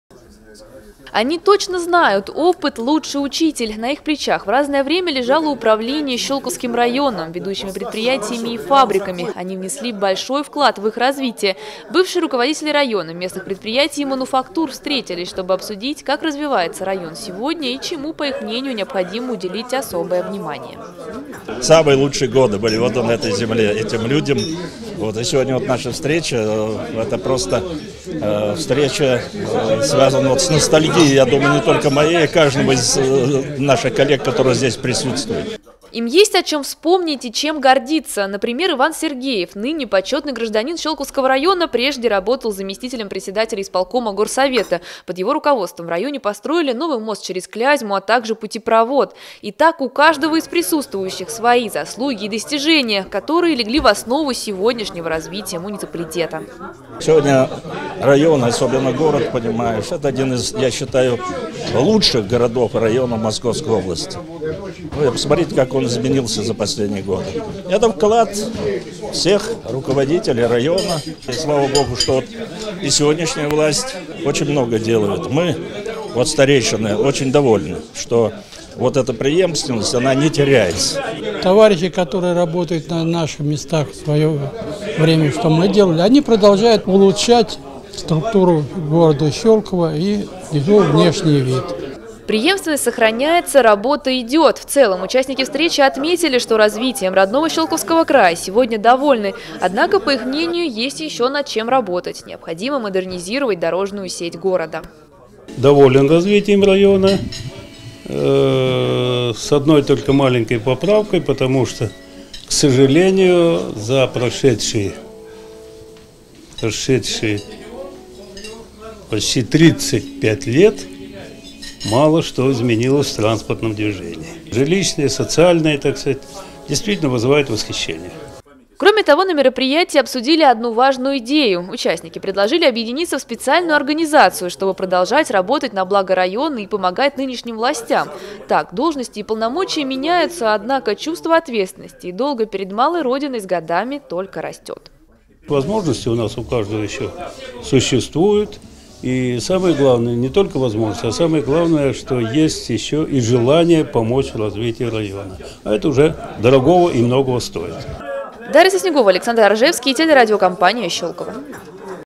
Бывшие руководители района и местных предприятий провели традиционную встречу
Им есть чем гордиться. Бывшие руководители Щёлковского района и местных предприятий провели встречу. Они планируют объединиться в специальную организацию, чтобы продолжать работать на благо своей малой родины и делиться опытом с действующим руководством муниципалитета.